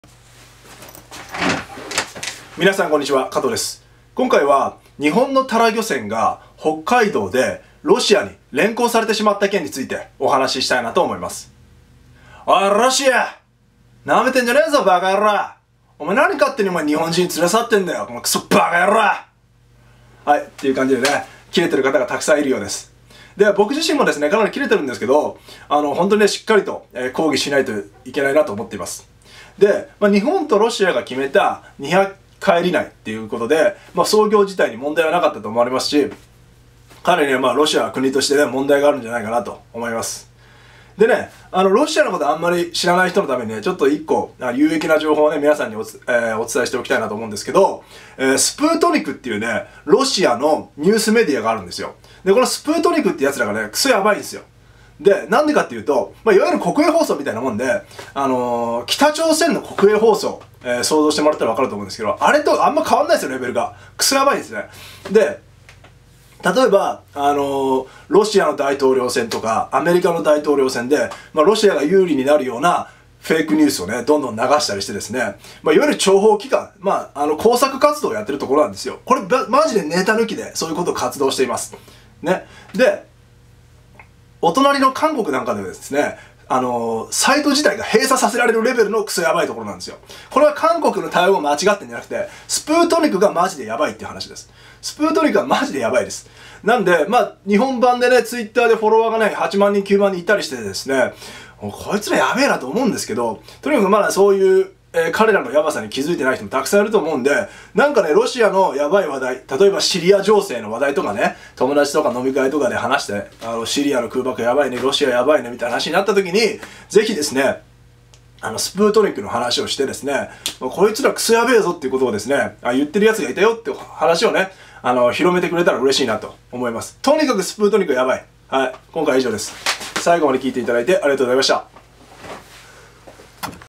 【速報】日本のタラ漁船がロシアに連行された件についてお話しします
おいロシア！！！ふざけんじゃねえぞ馬鹿野郎！！！！日本人を勝手に連行してんじゃねえ！！！！！！！！！！！！！！！

【ソース】
根室のタラ漁船をロシア側が連行　北海道庁